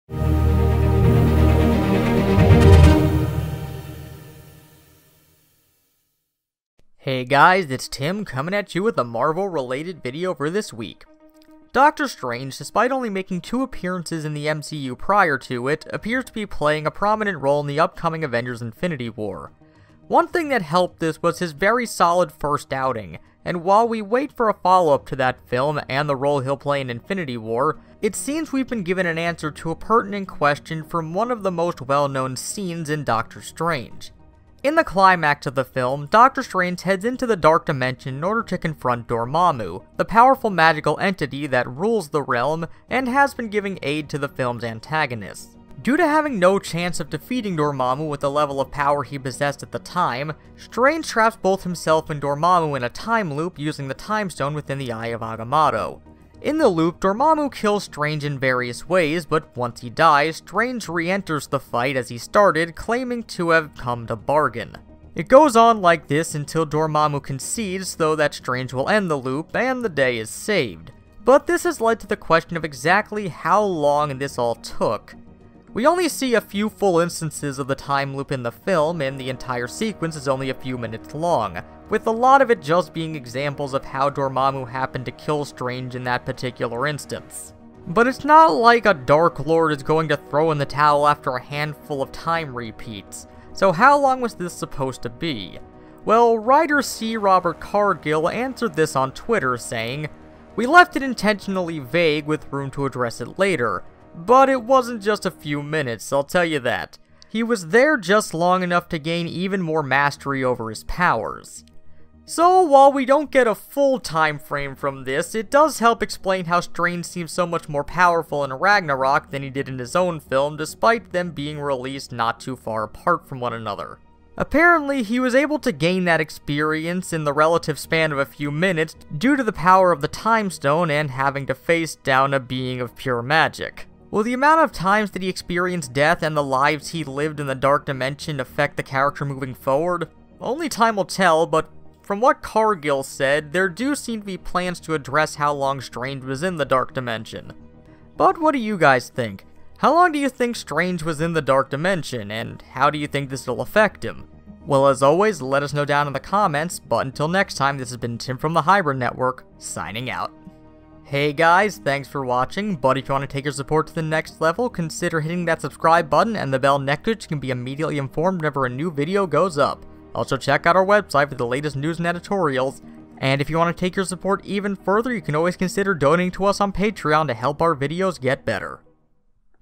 How Long Doctor Strange Was in the Dark Dimension Revealed | Avengers Infinity War
Just how long did Doctor Strange try to escape the Dark Dimension? Well we finally have an answer from screenwriter C. Robert Cargill...